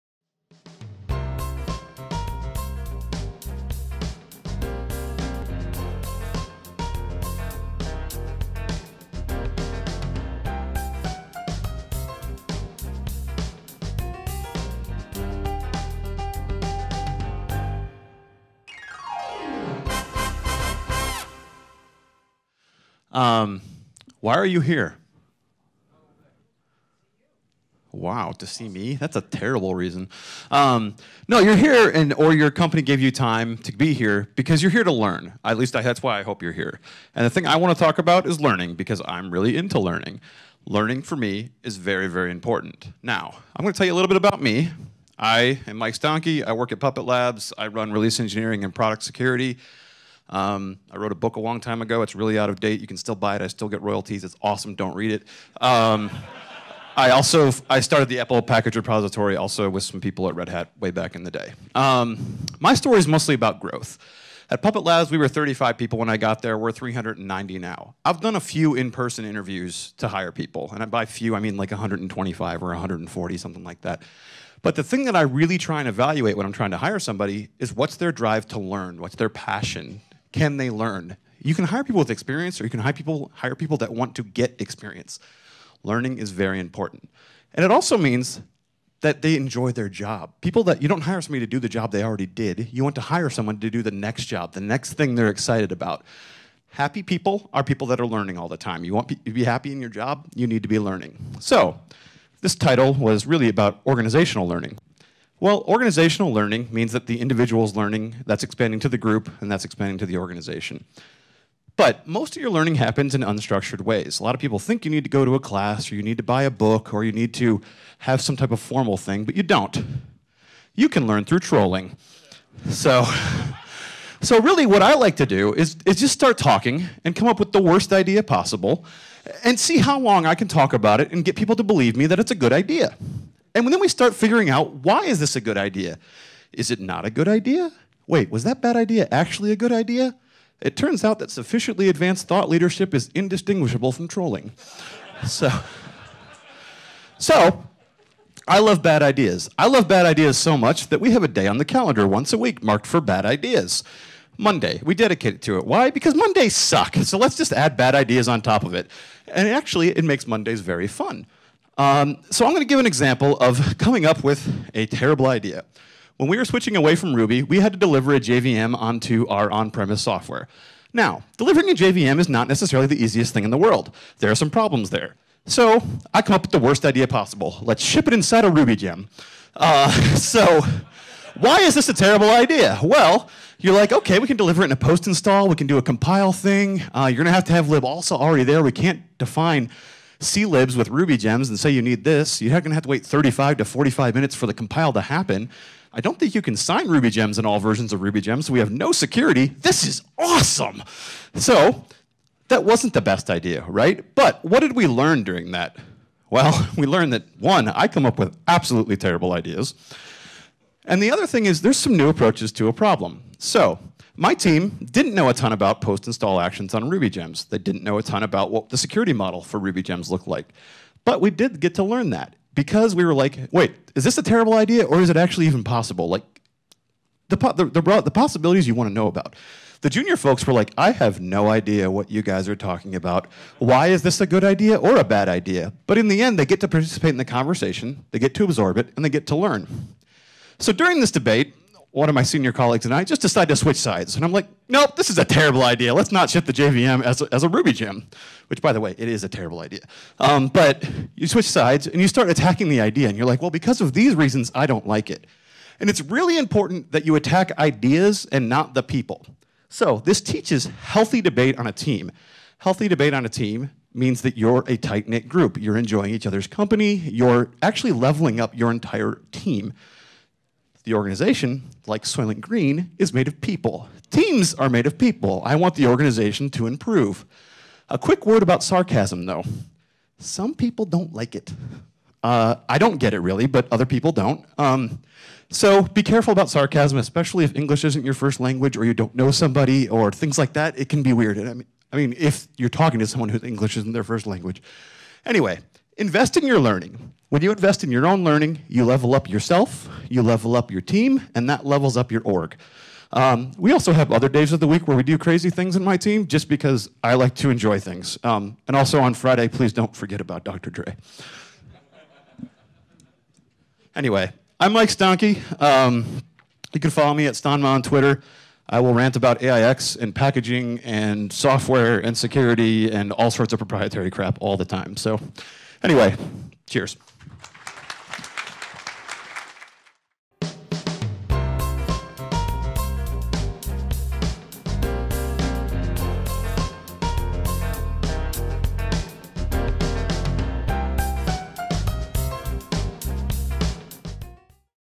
DevOpsDays Chicago 2015 - Organizational Learning Through Trolling by Michael Stahnke
Organizational Learning Through Trolling by Michael Stahnke